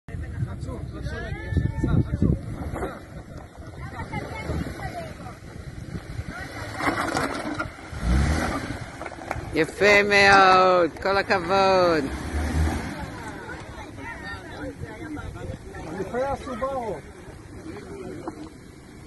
סיכום טיול ג'יפים לארץ הנבטים - 8/1/22
הדריך: אמיר נחומי